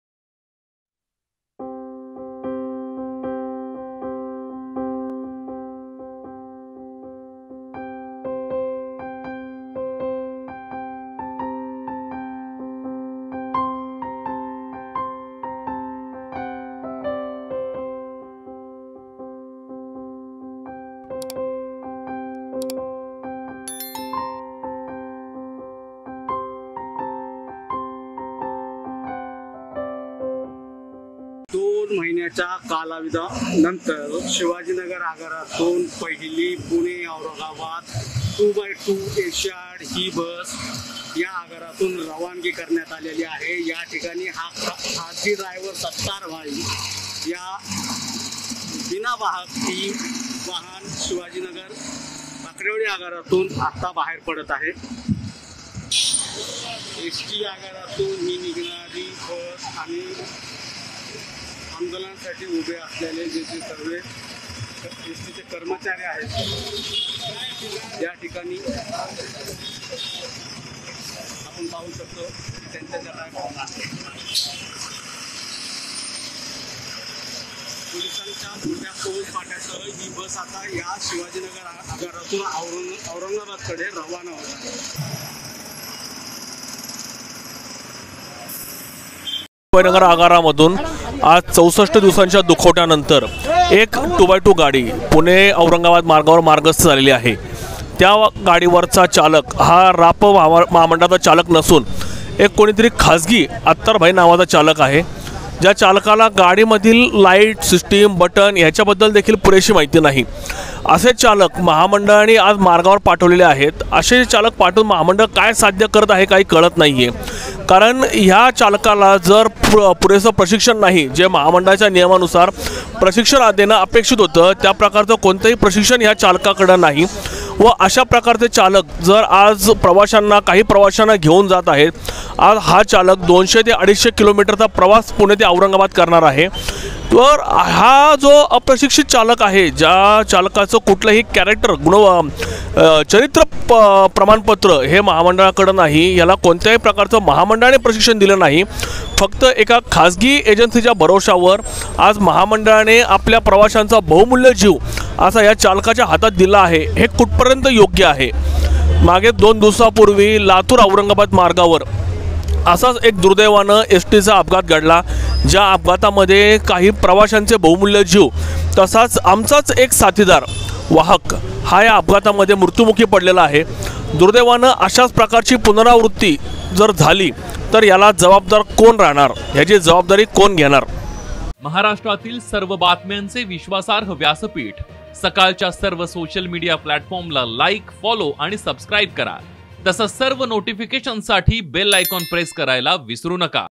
ST Strike l शिवाजीनगर आगारातून 64 दिवसानंतर सुटल्या 4 बसेस l Sakal
वाकडेवाडी विलीनीकरणाच्या मागणीसाठी एसटी कर्मचाऱ्यांचा संप सुरू आहे एसटी कर्मचाऱ्यांचा संप आज 64 वा दिवस सुरू आहे पुणे औरंगाबाद पुणे मुंबई अशा चार एसटी टू बाय टू आज आगारातून सोडण्यात आल्या भाडेतत्त्वावर घेतलेल्या  चालकांकडून ही गाडी आज रवाना करण्यात आली व्हिडिओ प्रमोद शेलार